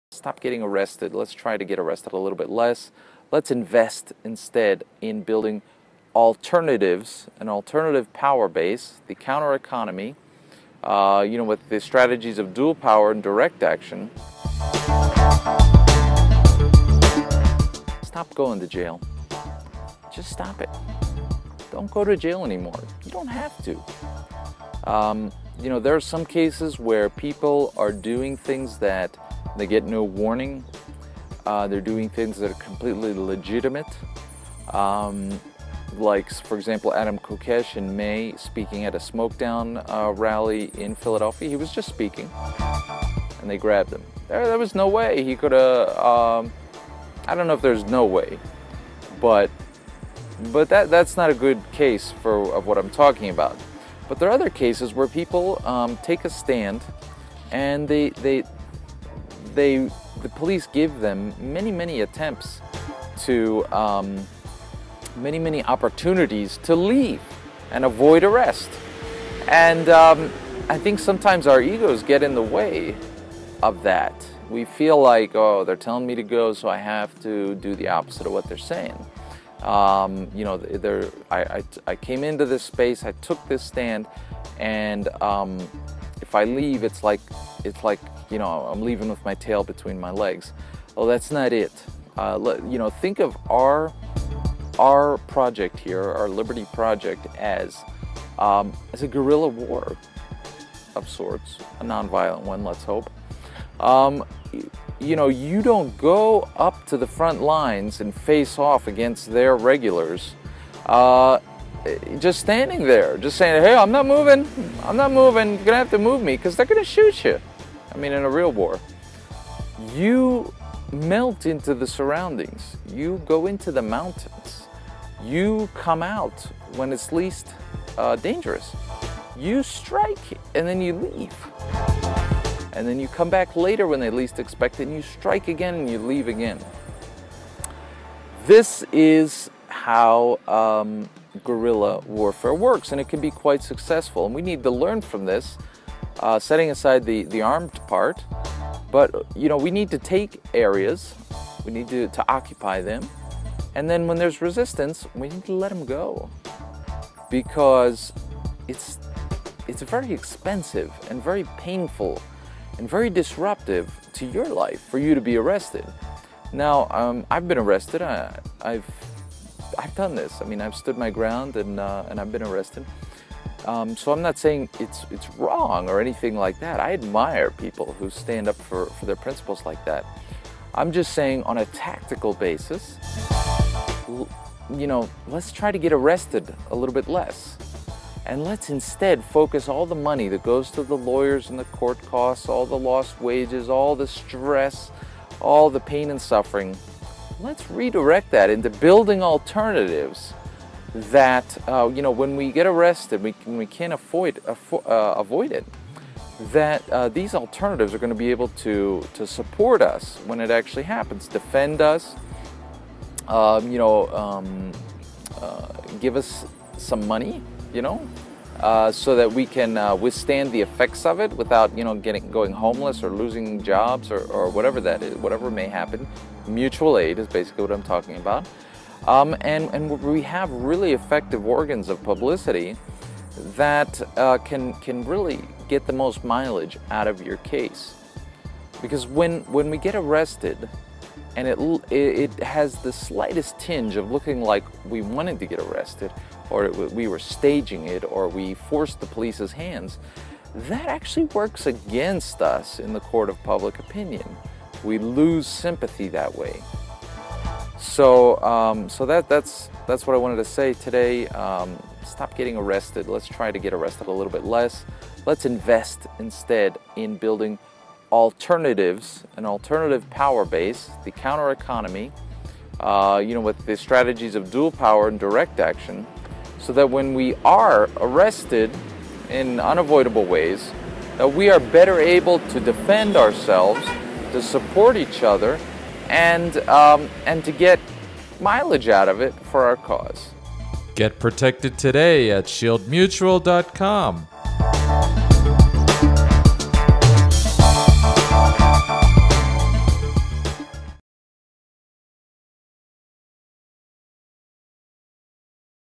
Stop Going to Jail!
We don't need to go to jail in order to be effective activists. In fact, quite the opposite is true.